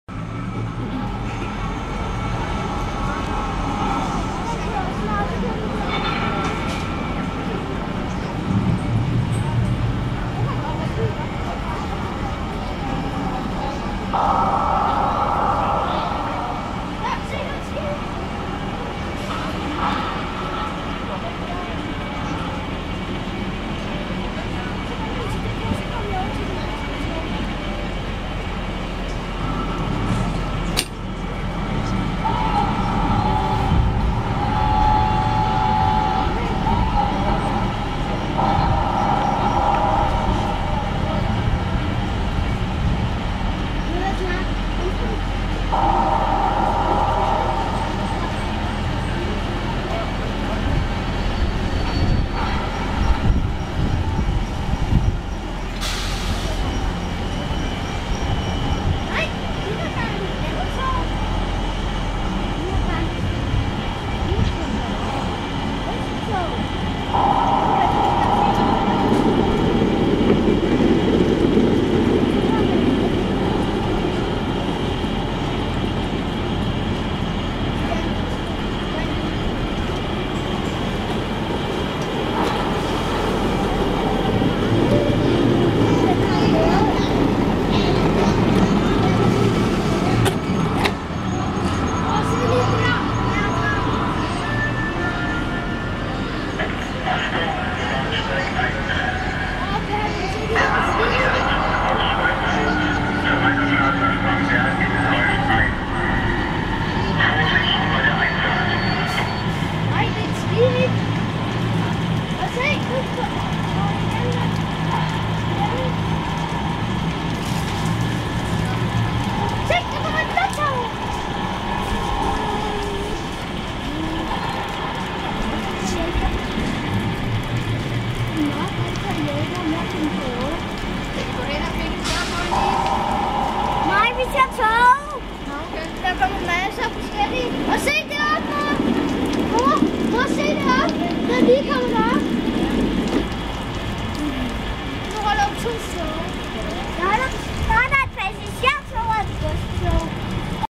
Lift Lock at Legoland
In Legoland's Minicity trains, ships and cars move. And they even have a working ship lift.